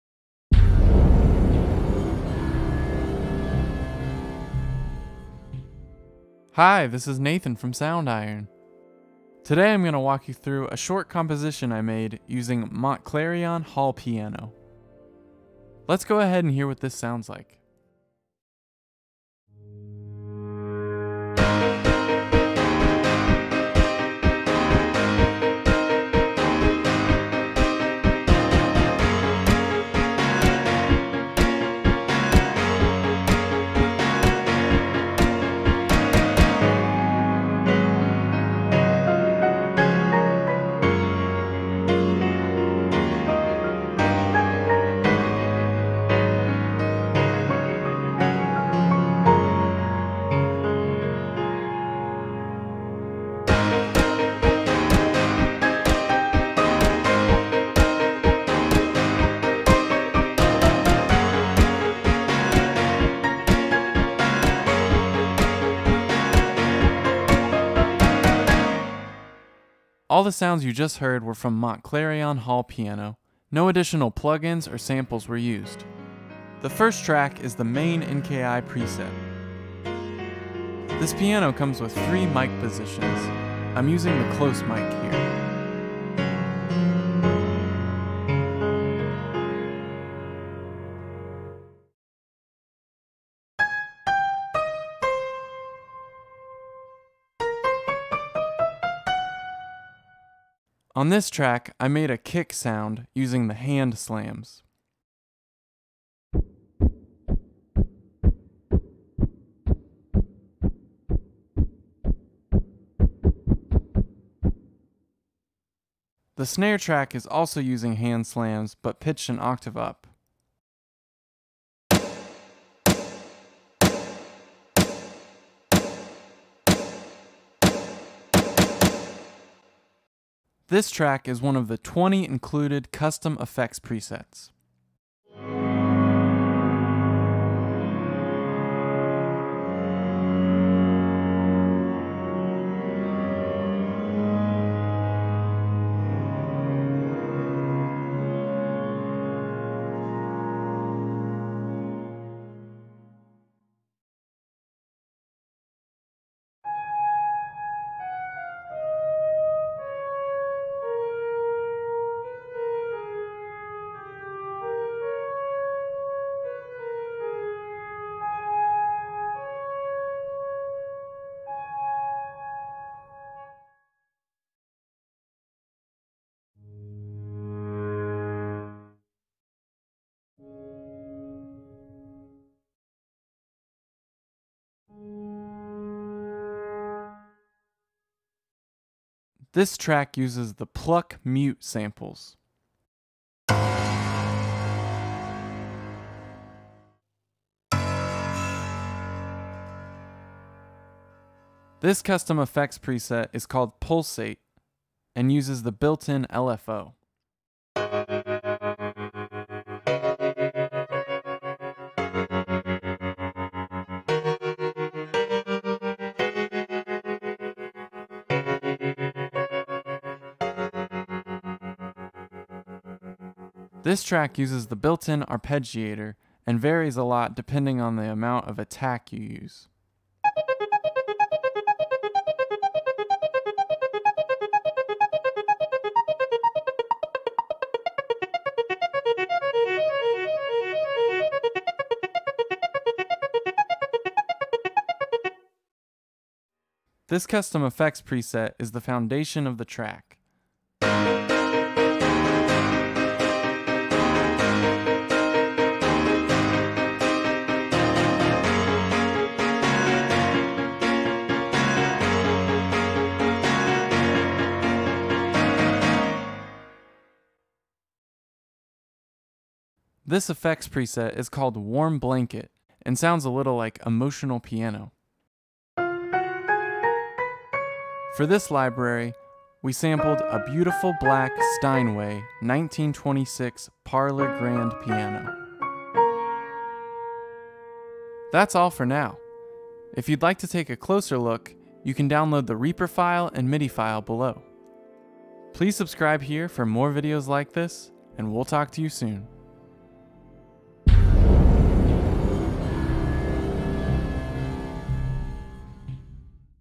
Composing Using Montclarion Hall Grand Piano With Nathan Boler
In this video Nathan Boler walks you through his short composition using only the sounds from Montclarion Hall Grand Piano.